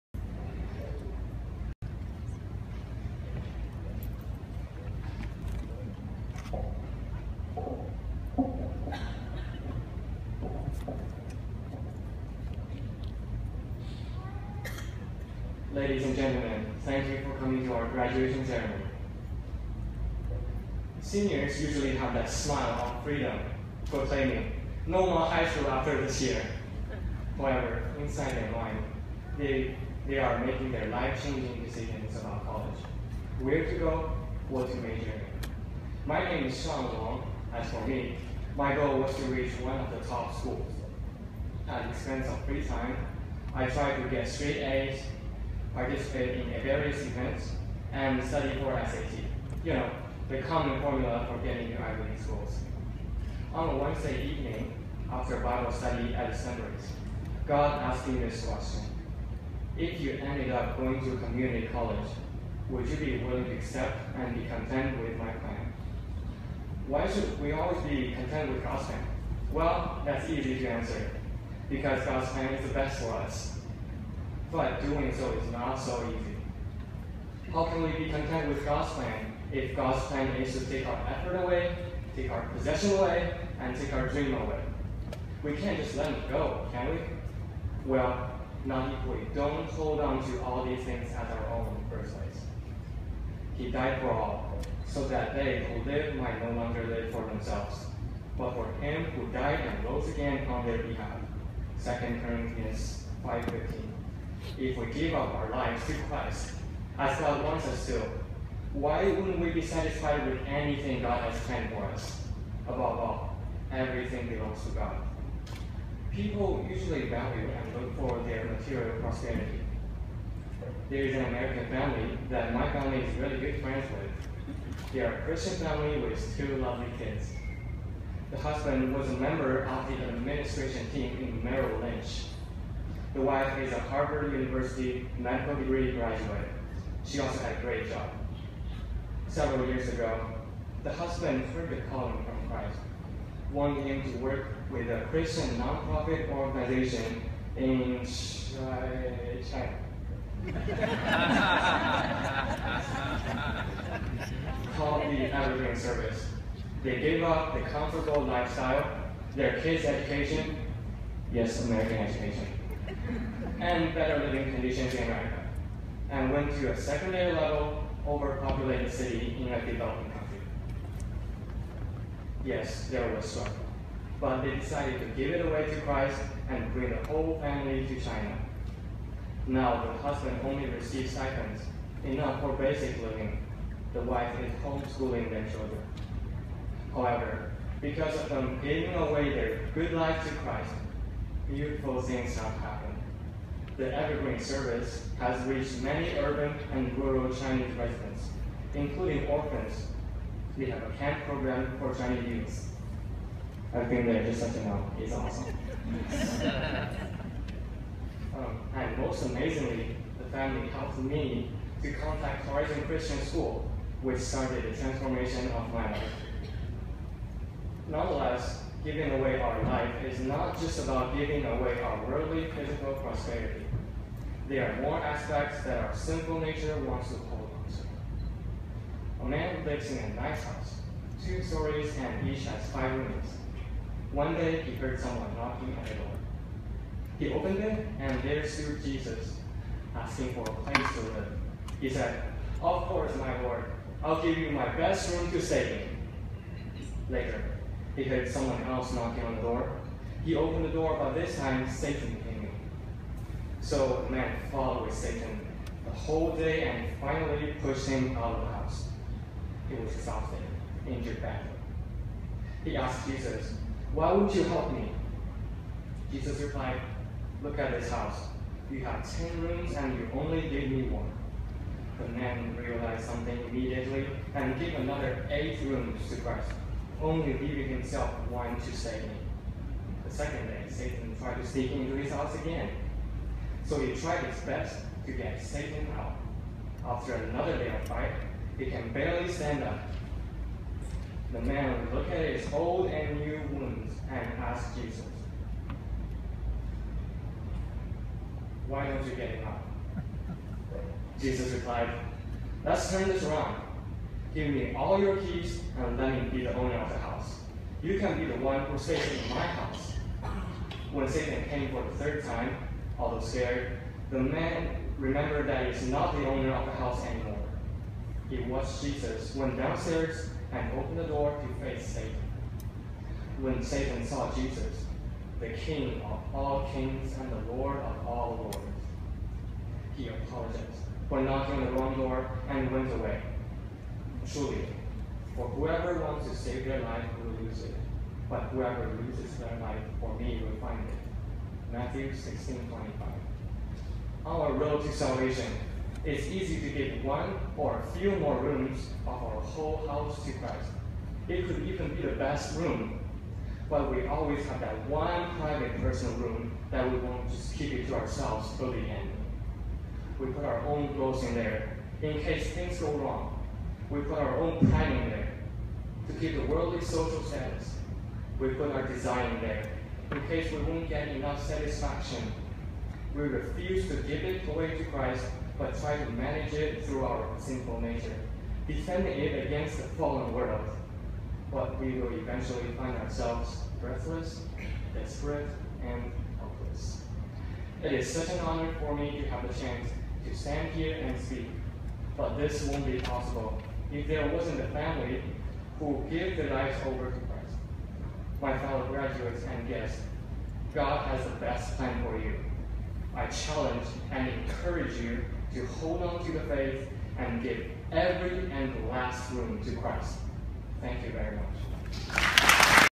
Shaun Graduation Speech
Shaun Wang spoke at the Horizon Christian School Class of 2015 Graduation Ceremony as one of the Horizon Image Bearer.
Topic: Give your life away to Christ.